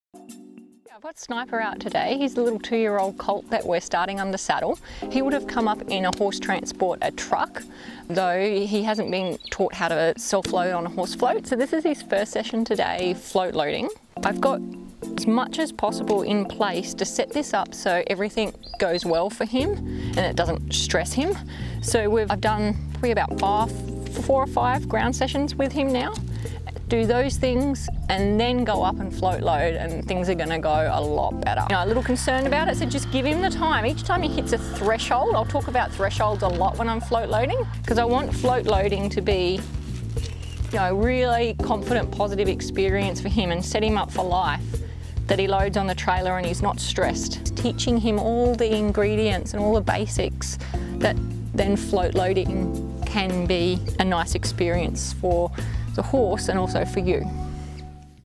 Teaching a 2-Year-Old to Load on the Horse Float for the First Time
In this informative video, Angie guides you through the essential steps of teaching a 2-year-old horse to load onto a trailer for the very first time. Trailer loading can be a challenging experience for both horses and their handlers, so it’s crucial to establish a strong foundation of groundwork skills before attempting this process.

Before you begin loading your horse, ensure that they are comfortable with basic groundwork exercises. This includes backing up out of your personal space, responding to pressure by moving forward, and desensitising to tools such as a stick and flag or string. Additionally, your horse should be able to move their hindquarters and lunge around you, as well as navigate obstacles with confidence. Mastering these foundational skills will significantly enhance the loading experience and help create a positive atmosphere for both you and your horse.

Throughout the video, Angie emphasises the importance of understanding your horse's thresholds and the concept of release during the loading process. By working patiently and compassionately with your horse, you can help them feel more secure and relaxed when loading onto the trailer. This approach fosters a stronger bond between you and your horse, setting the stage for successful loading experiences in the future.

Join Angie as she shares her insights and techniques for making trailer loading a smooth and enjoyable experience for your young horse!